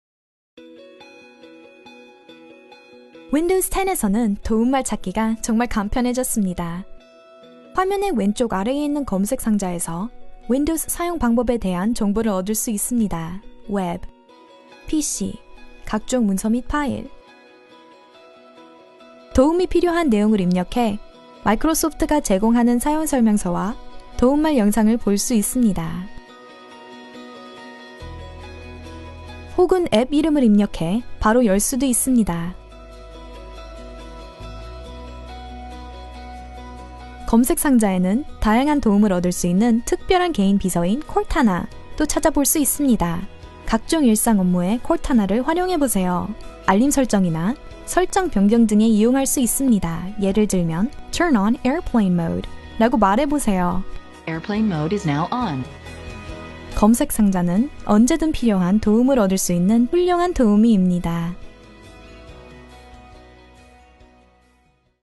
Windows 10 : 도움말 및 검색 상자 활용법
Windows 10에 대한 도움이 필요하세요? 작업 표시줄의 검색 상자에 키워드나 질문을 입력하면 Microsoft 도움말을 찾을 수 있습니다.